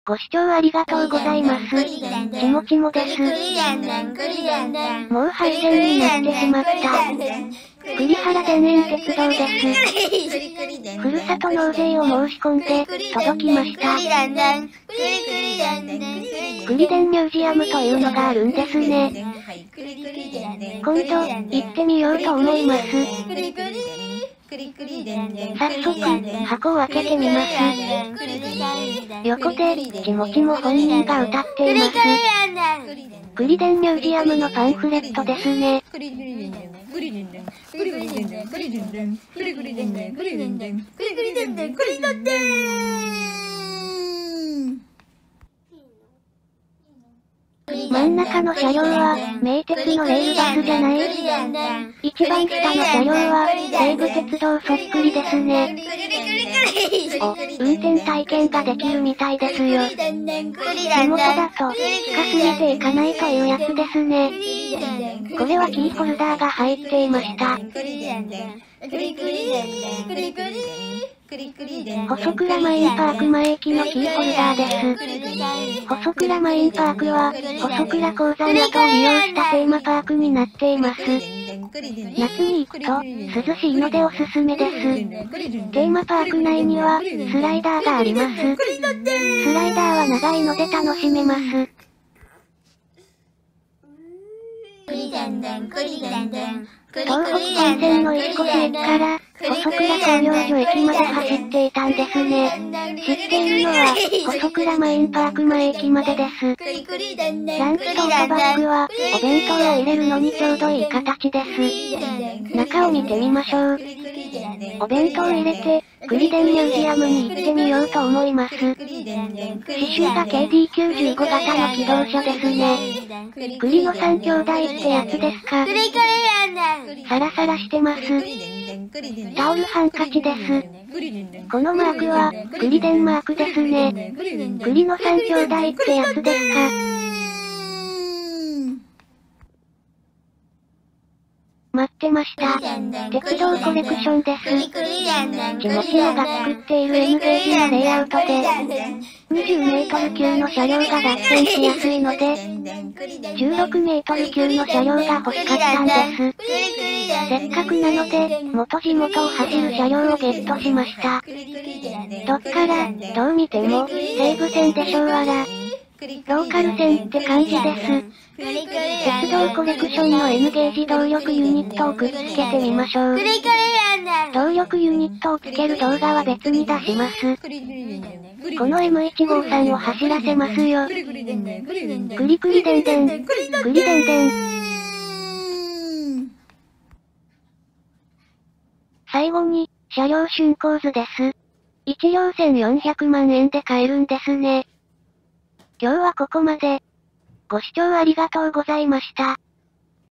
【鉄道模型】くりでんの自作歌付き、くりはら田園鉄道(廃線) M153 TOMYTEC / N-Gauge
くりはら田園鉄道(廃線)のM153の鉄道模型です。
普通に買おうと思っていたのですが、ふるさと納税で見つけましたので、ふるさと納税で鉄道模型が届きました。
16m級車両なので、今作っているＮゲージのレイアウトで走らせてみようと思います。

栗原電鉄・くりはら田園鉄道は、東北本線と接続する石越から細倉マインパークまえ駅までを結んでいた鉄道です。今は廃線となっています。
終点の細倉鉱山で鉱石や加工したものの輸送を主な目的としていました。
細倉鉱山閉山後は旅客輸送のみとなり、1995年の電化廃止時に栗原電鉄から、くりはら田園鉄道に改称されました。
2007年3月31日をもって全線廃止となりました。
一部の車両は旧若柳駅にて保存されています。
保存している車両は、定期的に乗車会が開催されています。
2017年4月には、「くりでんミュージアム」が開館しました。